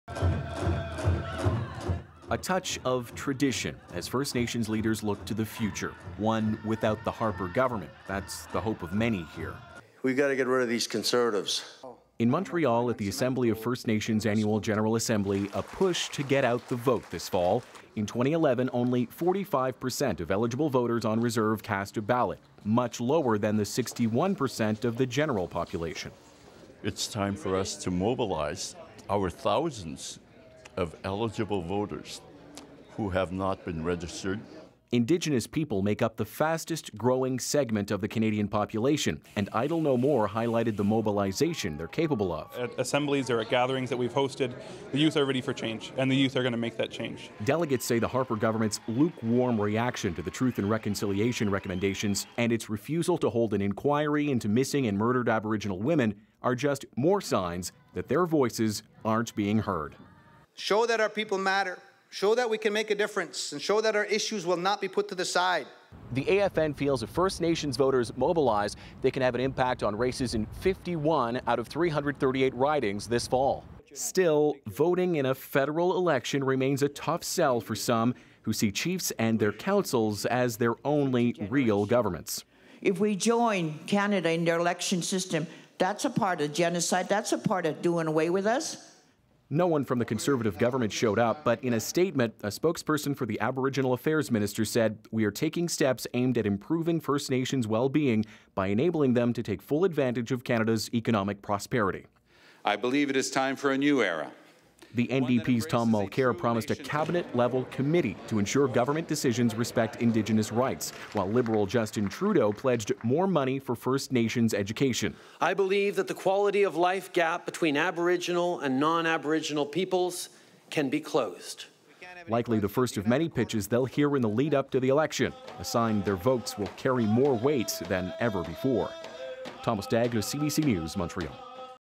AFN General Assembly discusses the federal election
There is a push to get indigenous Canadians to vote in the fall election.